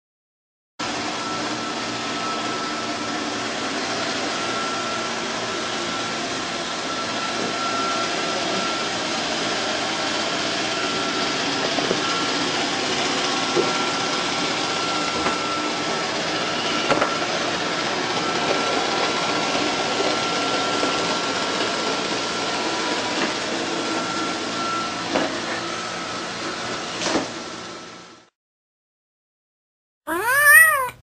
Cute cat does housework riding a robovac
Visit the official CatPowa.com website for the latest funny cat videos
This cat knows how to get chores done in style, riding stag on the robovac (that's a robotic vacuum cleaner to you and me) until he comes unstuck...